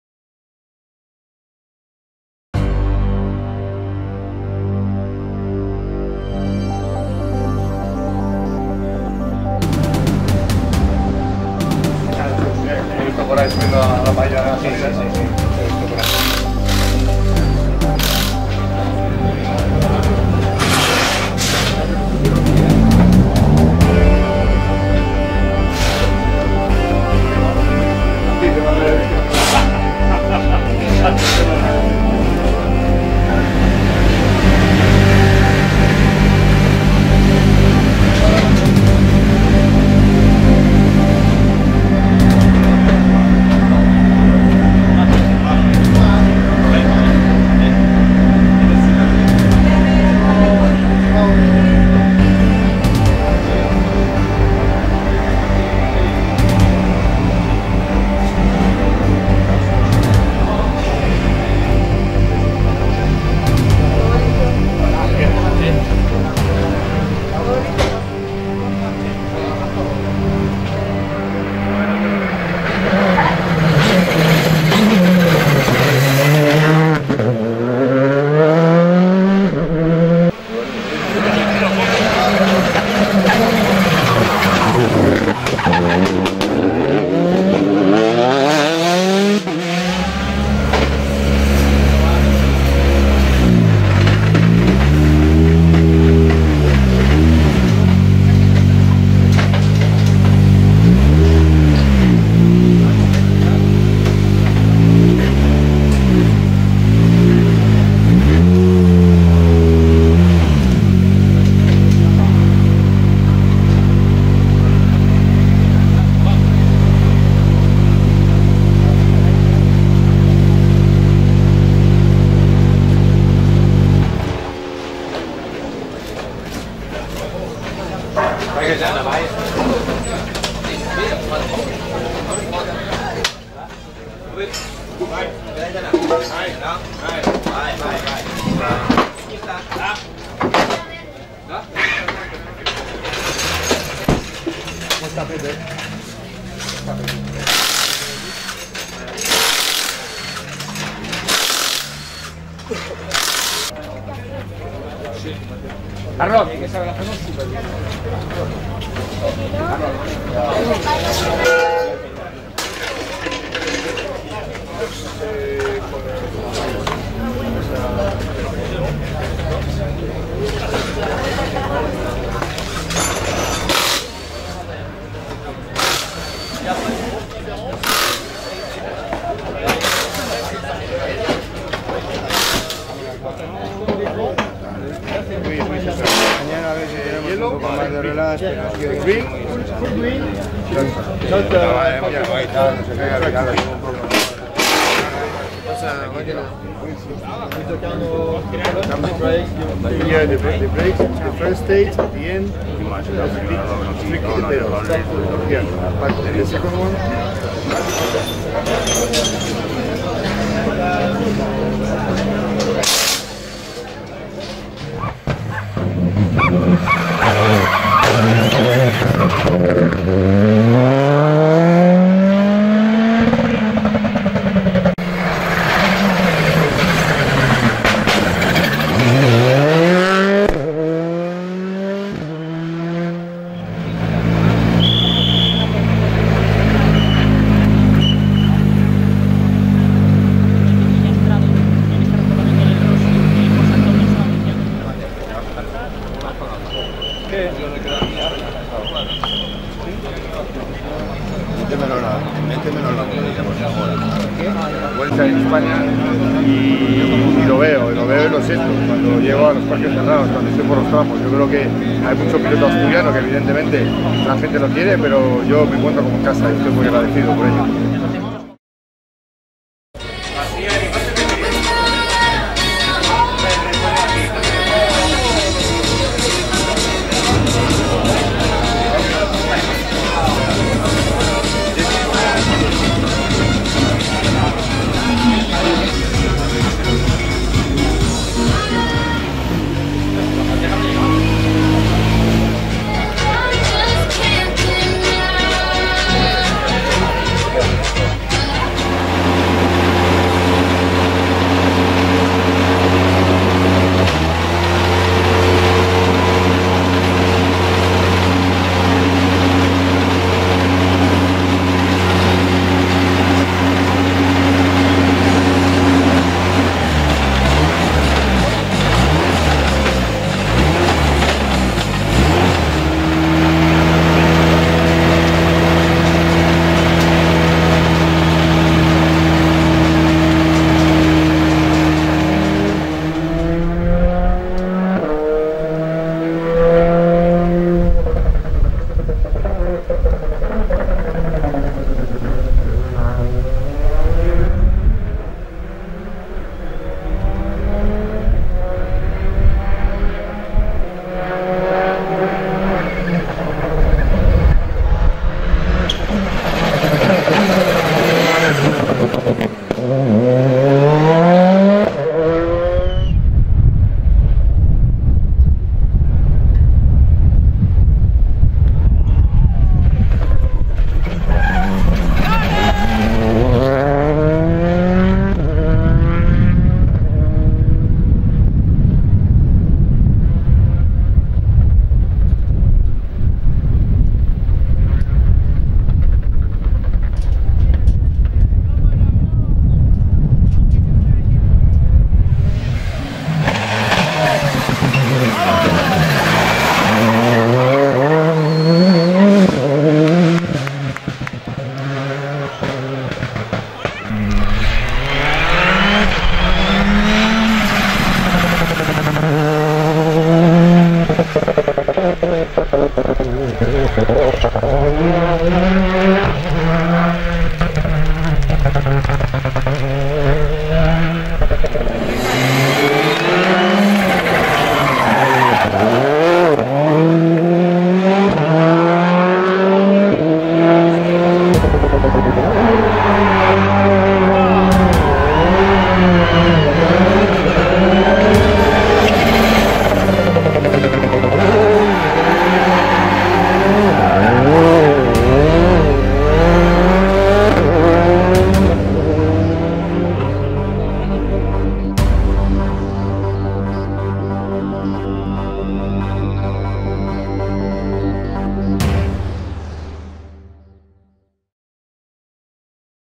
CHUS PURAS / PURASangre de los Rallyes (Parte 2)
Tras varios años apartado de los rallyes, en 2019 reapareció en el Rallye Festival Hoznayo a los mandos de un Citroën C3 R5 con Carlos Dorado a la derecha.
Y no quedó ahi, dos nuevas participaciones en el Princesa de Asturias y en el Santander Cantabria.
En el primero tras un pinchazo en la ES1, finalizaron en la posición 9 y en el de casa, un vuelco en el Shakedown complicó la cosa terminando en una meritoria posición 5.